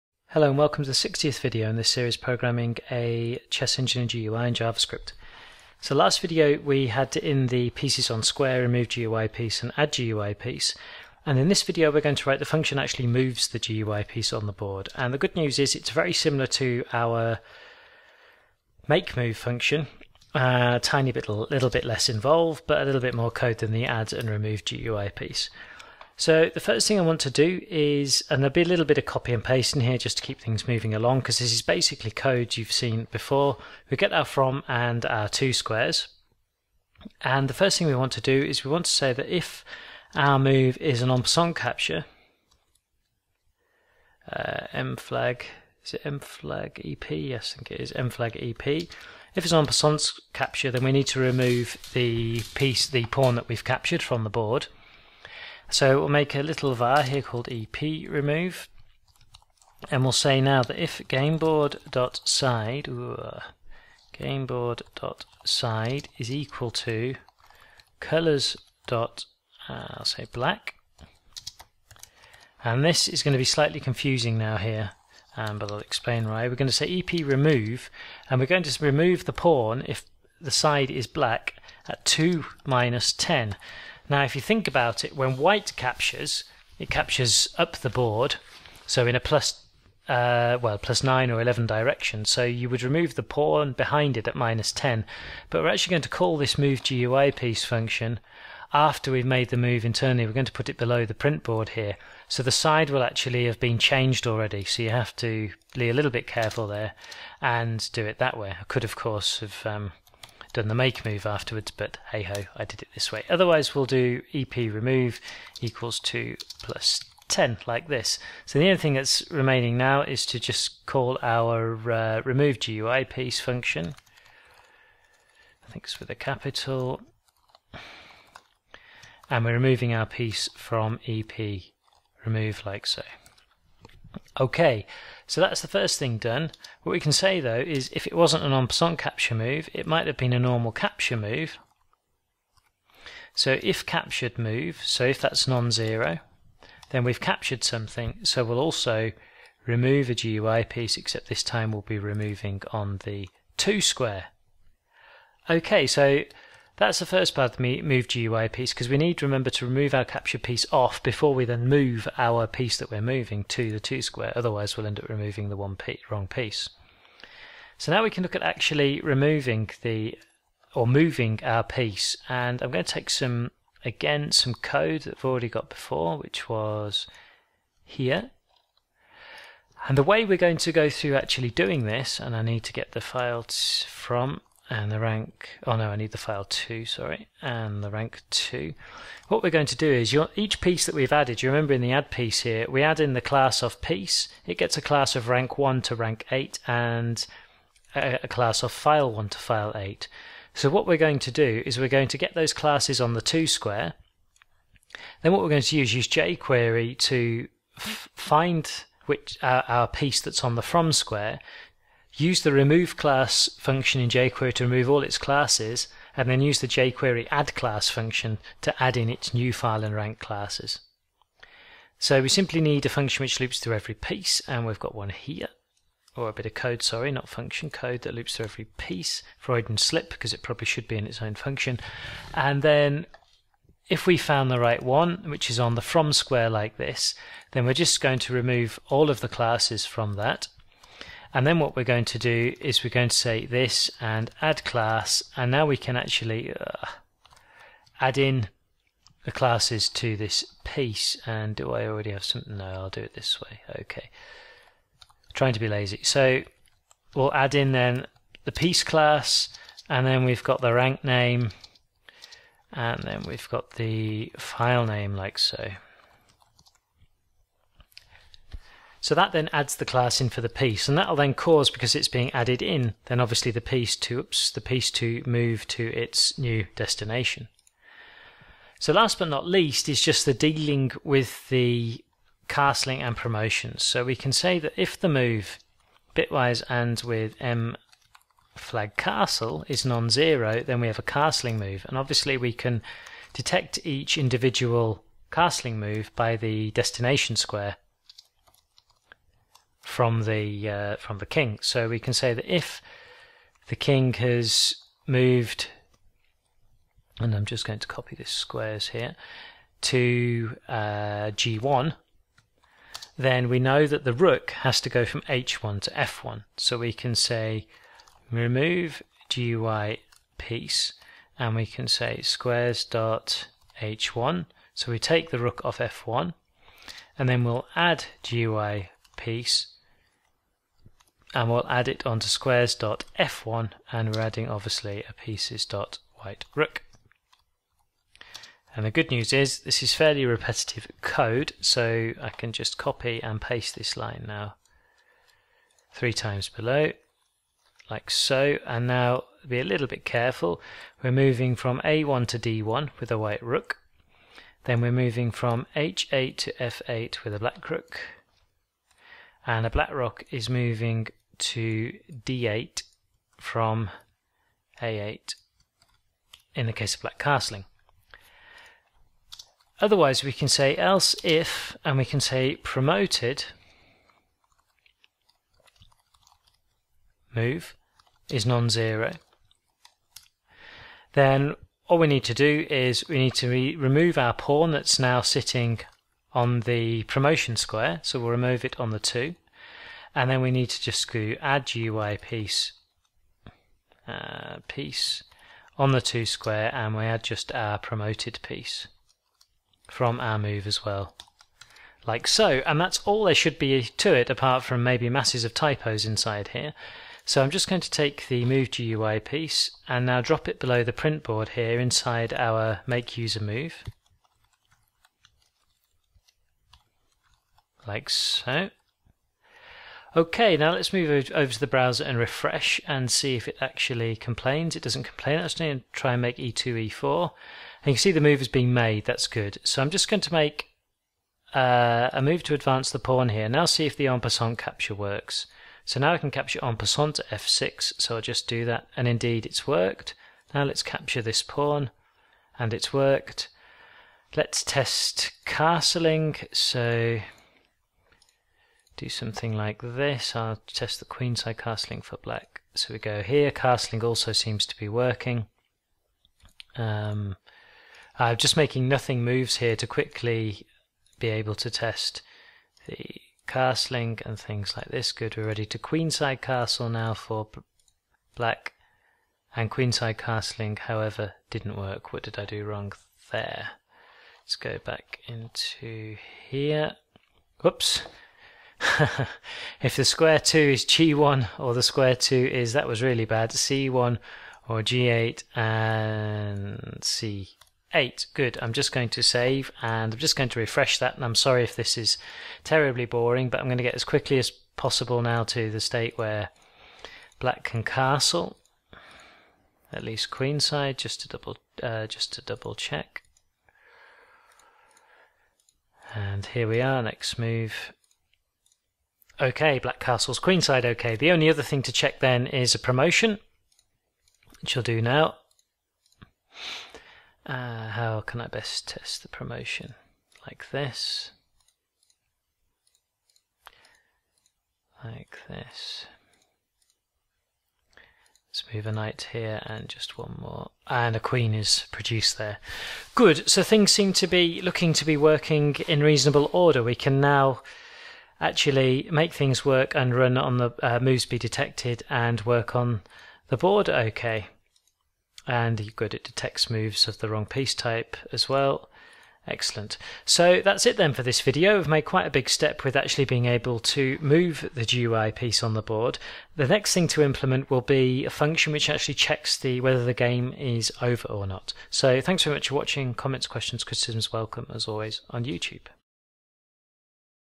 Programming A Chess Engine In Pure Javascript Part 60 - Bulding the (simple) GUI - Move Piece
A tutorial series programming a chess engine in Javascript